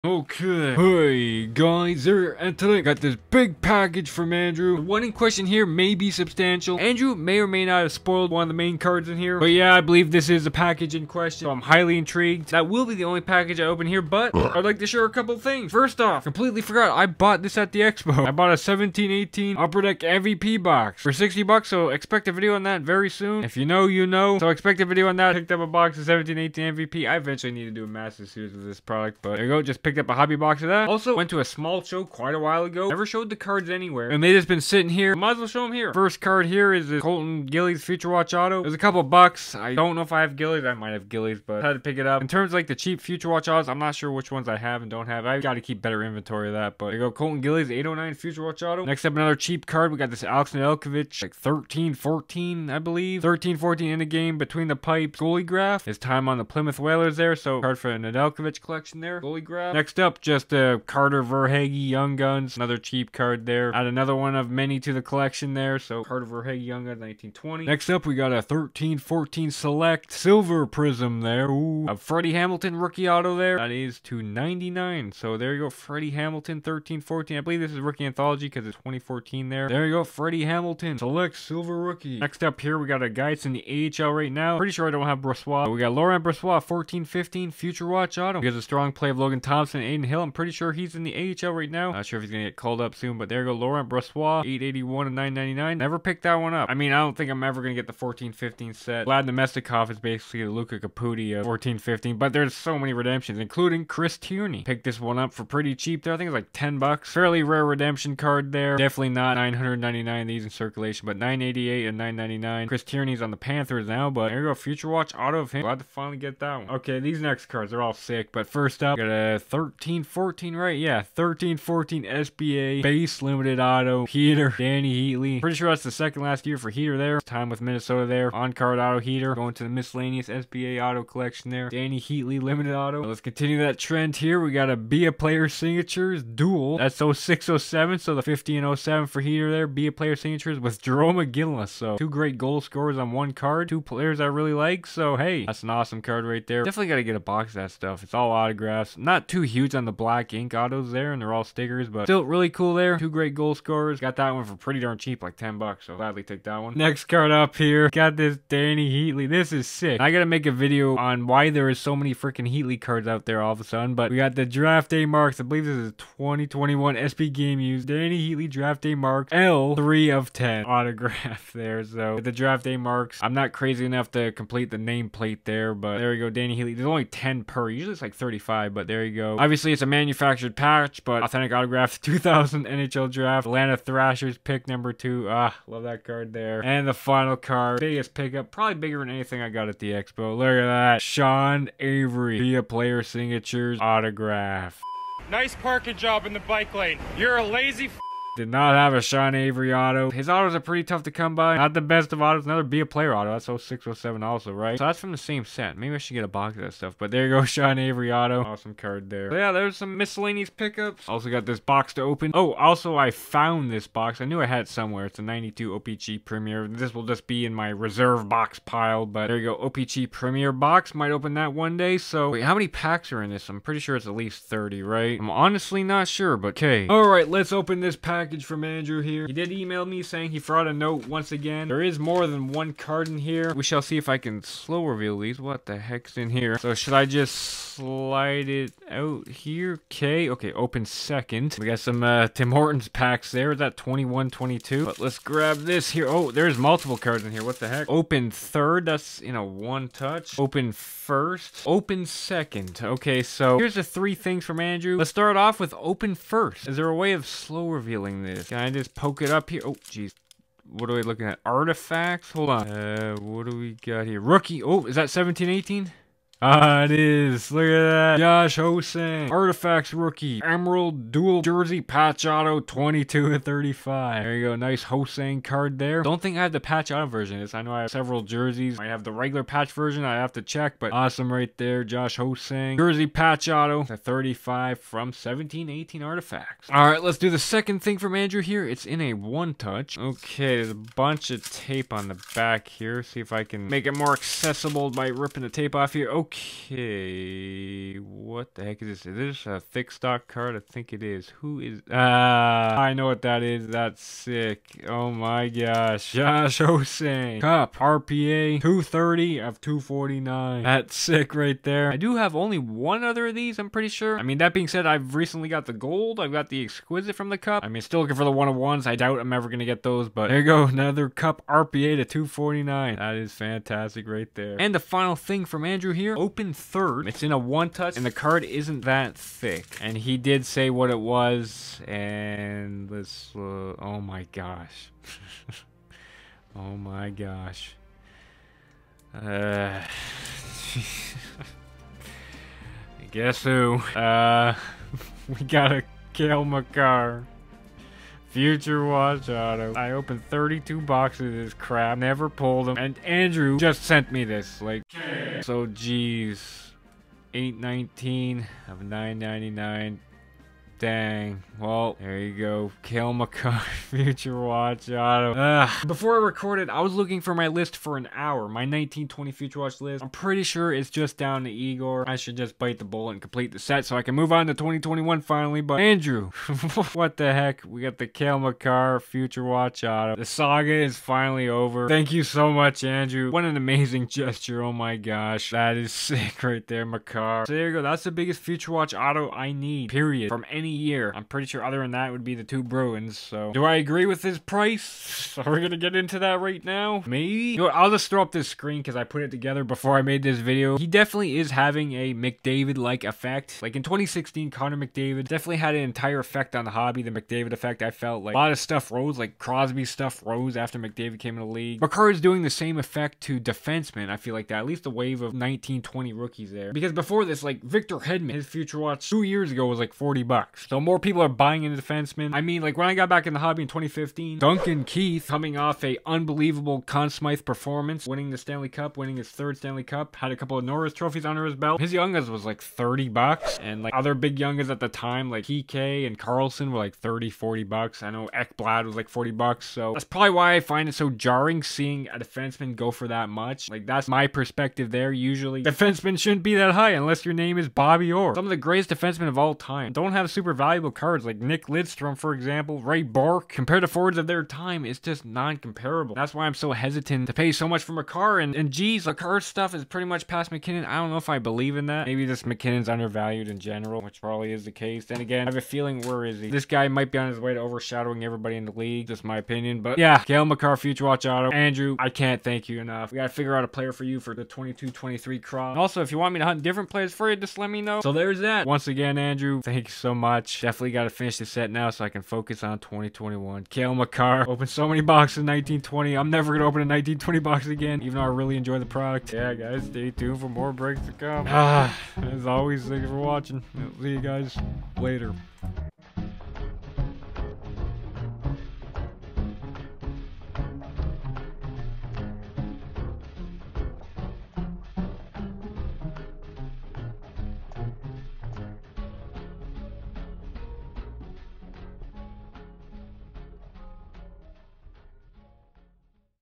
THE CRAZIEST CARD ANYONE HAS EVER SENT ME
Here is Mail #106 featuring a special package from Andrew which includes my most elusive card.

zeeree
St Catharines ON L2R 7P6
Canada